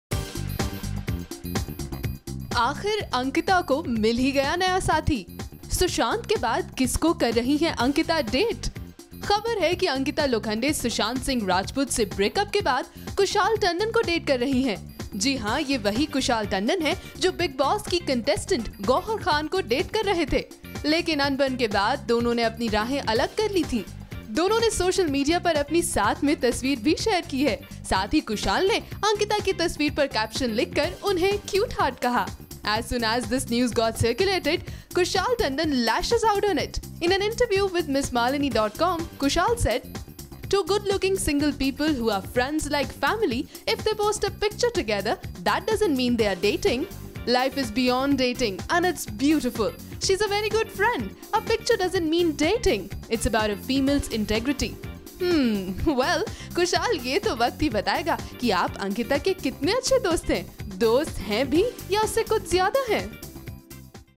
Kushal finally opens up on affairs rumours with Ankita Lokhande
Rumours were doing rounds that Kushal Tandon is dating Ankita Lokhande. Now Kushal opened up on the rumours and speaks on his relationship with Ankita Lokhande. Watch video to see What actor has to say on this.